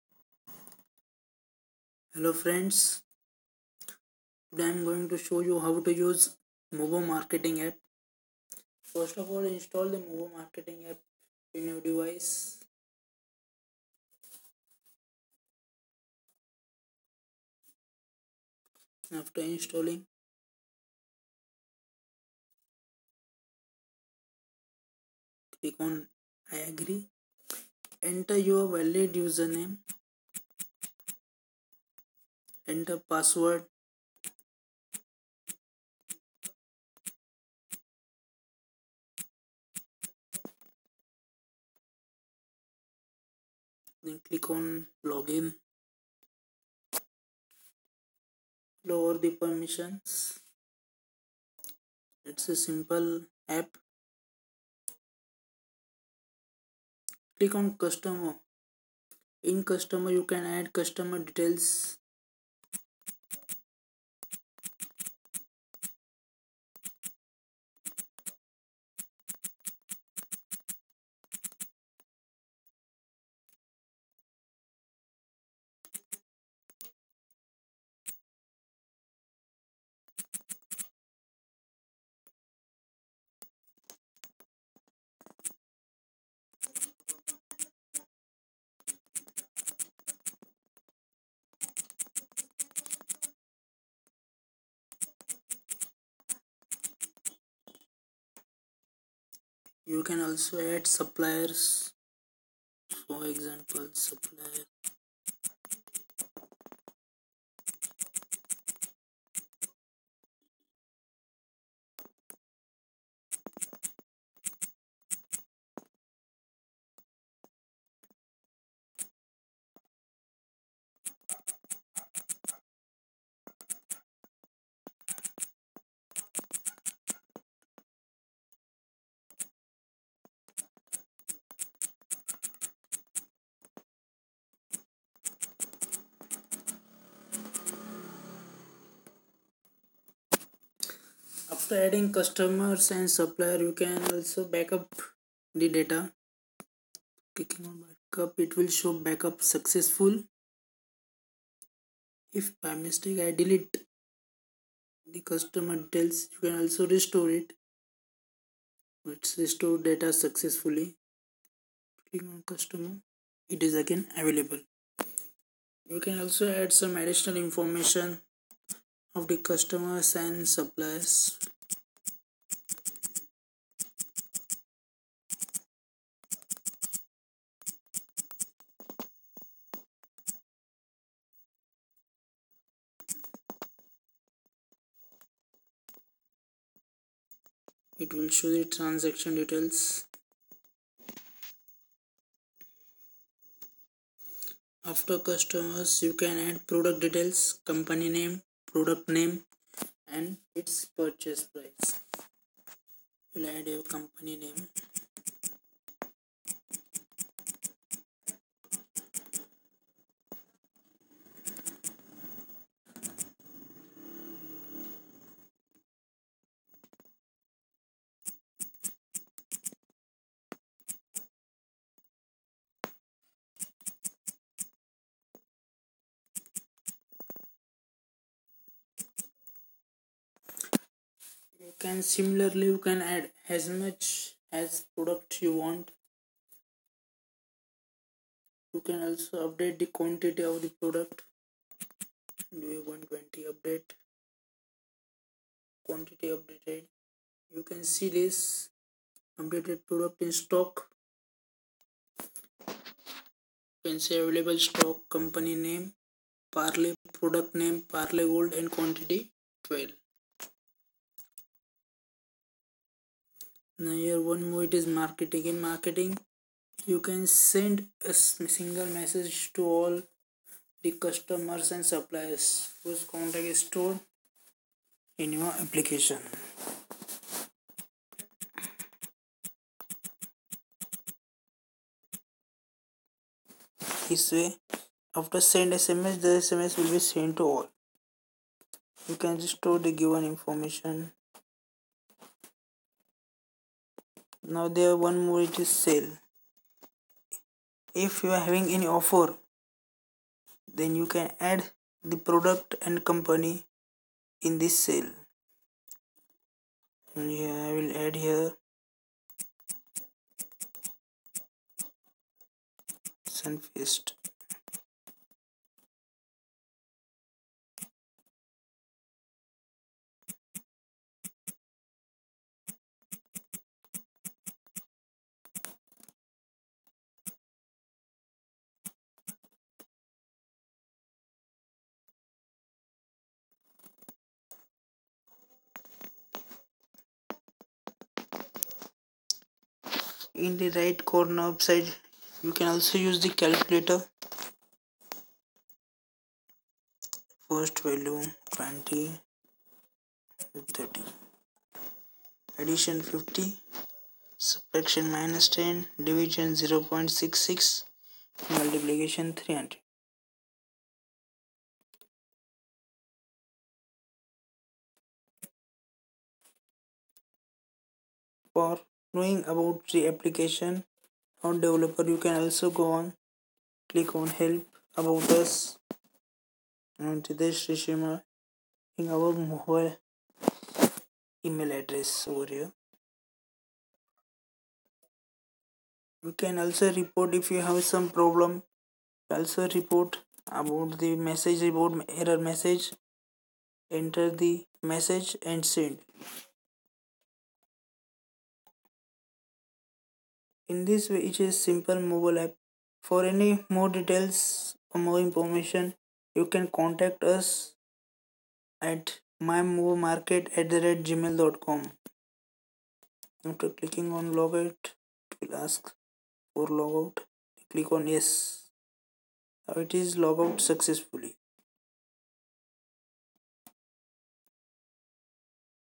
How to use MoBo Market App..[14-03-2018]
Mobo Market : It is the simple way to maintain Your own business .
Digital Marketing i.e. Marketing of product or any other is done with MoBo Market App...